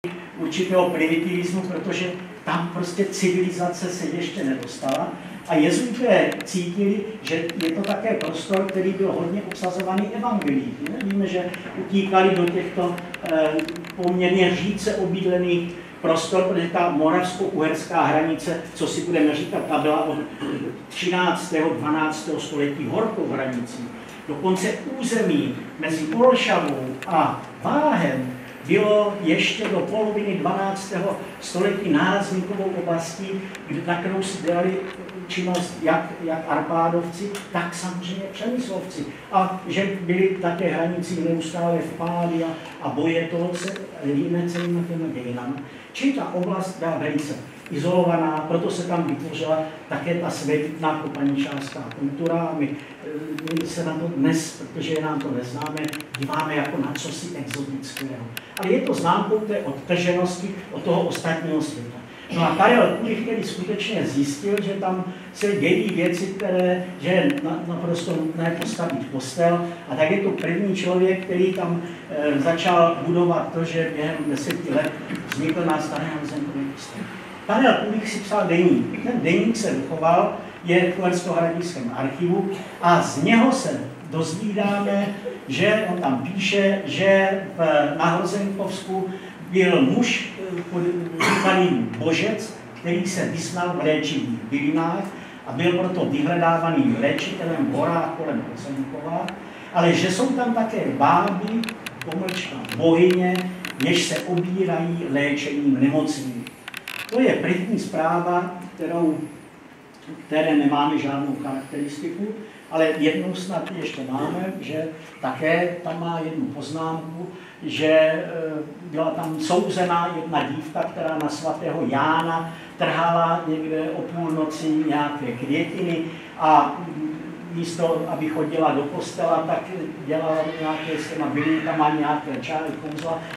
TRENČÍN-Velký zájem posluchačů při besedě s Jiřím Jilíkem, autorem knihy o Žitkovských bohyních
- 11.ríjna 2022, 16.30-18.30 hodin, společenský sál městské knihovny v Trenčíně